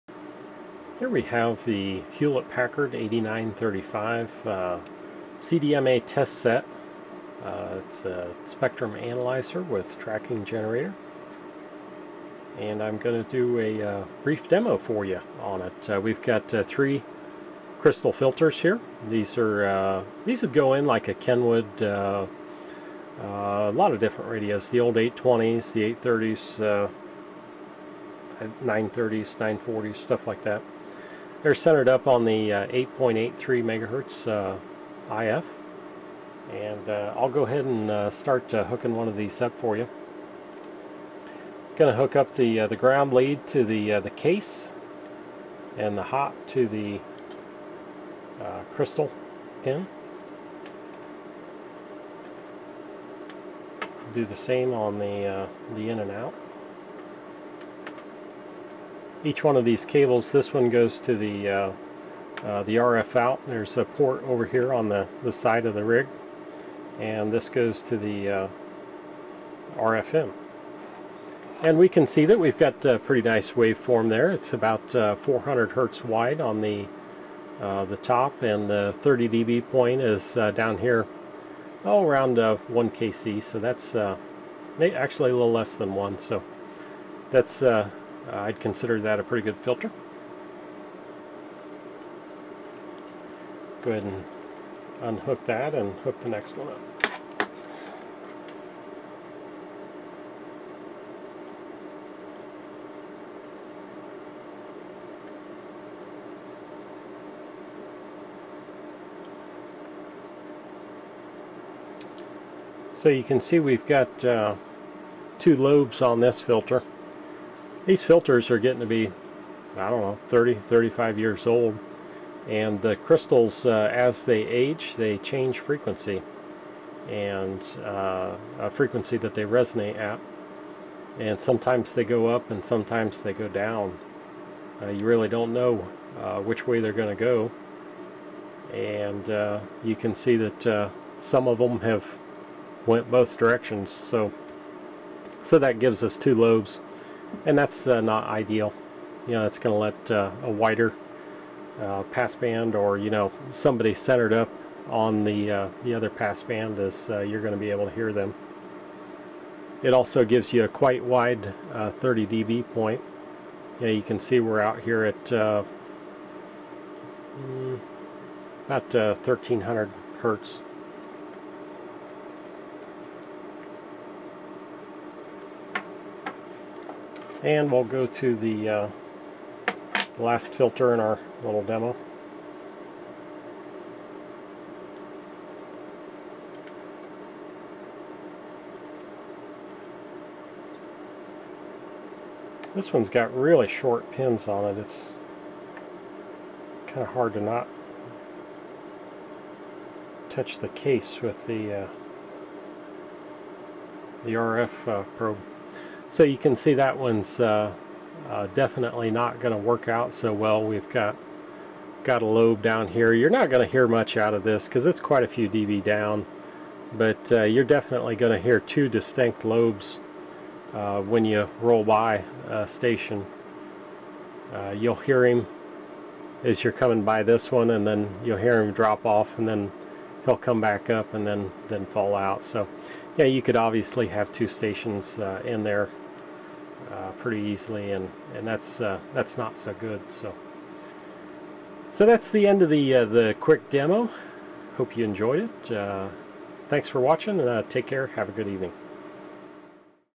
HP 8935 Demo
Short video showing the HP 8935 sweeping some 400 hz crystal CW filters.

Center Frequency 8.83 mHz
Span 5 kHz
500 Hz per Horizontal Division
10 DB per Vertical Division